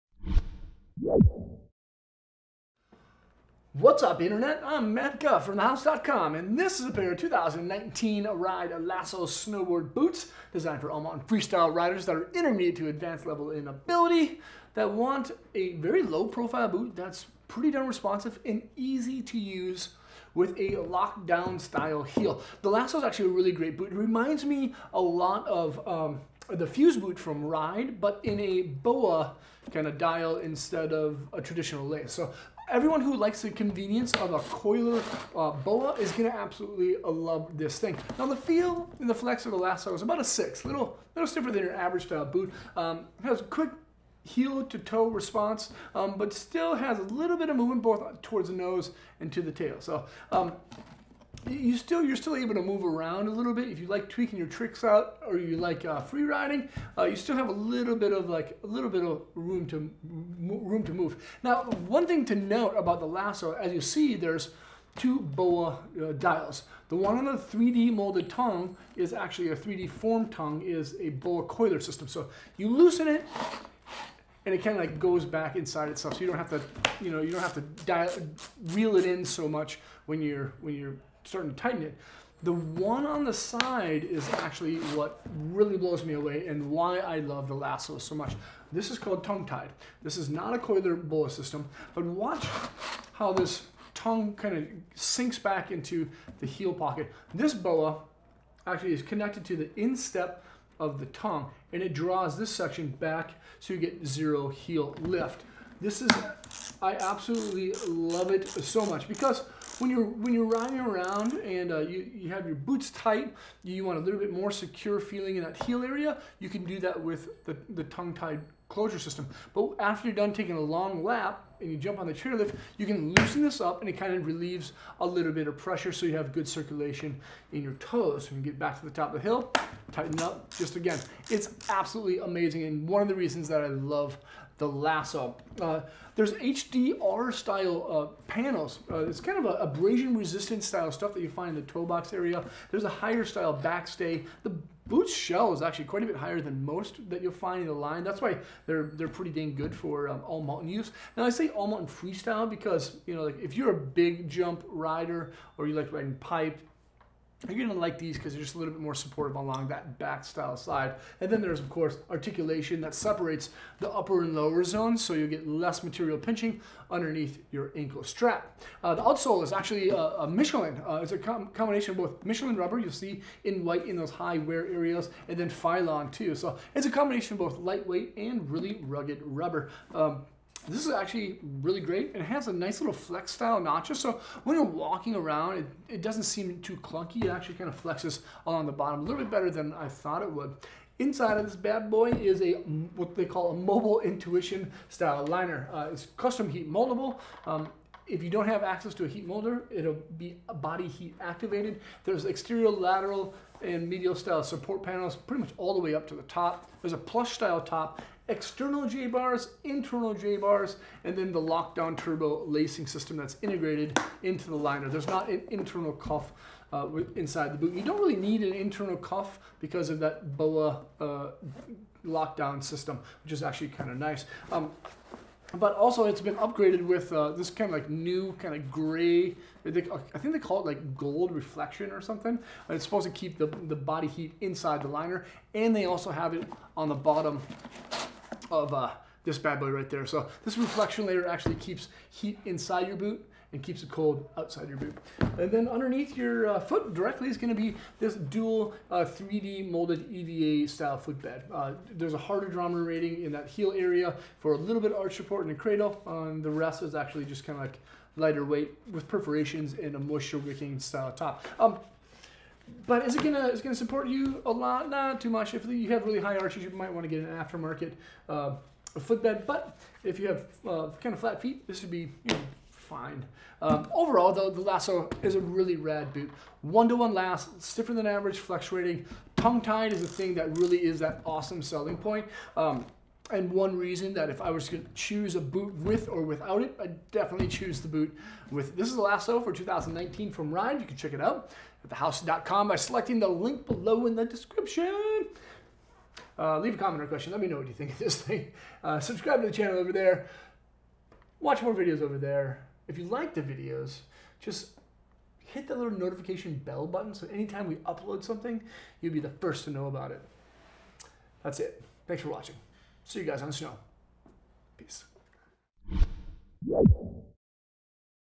2019 Ride Lasso Snowboard Boots Review
The 2019 Ride Lasso Snowboard Boots are an all mountain boot. It has a multiple BOA system - one designated for tongue closure. The outsole is lightweight and durable rubber.